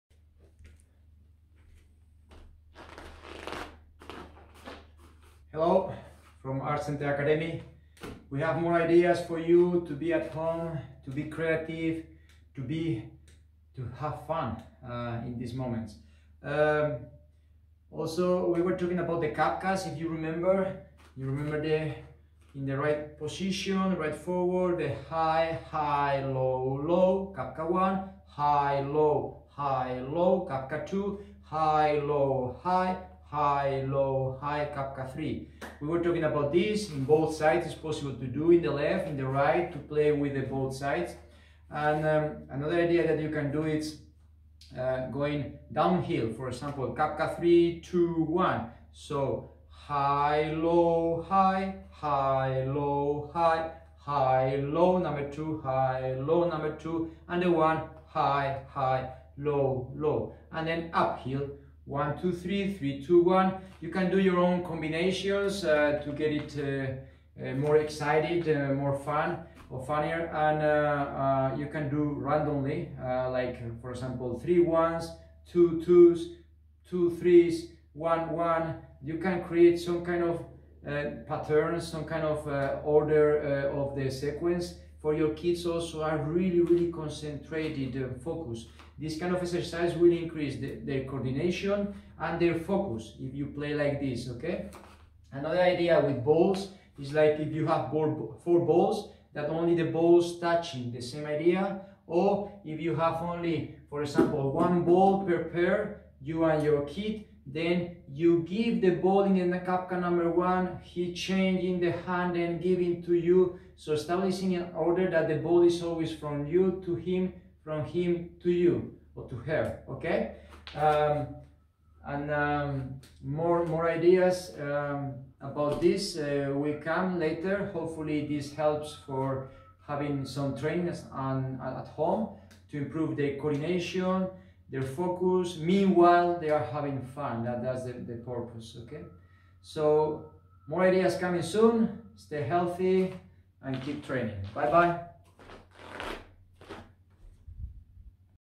Kali Majapahit Finland_ Kids ideas at home with balls part 2
More ideas about how to develop your kids coordination and focus with balls. Some basic movements like cabcas 1-3 are from Kali Majapahit martial art.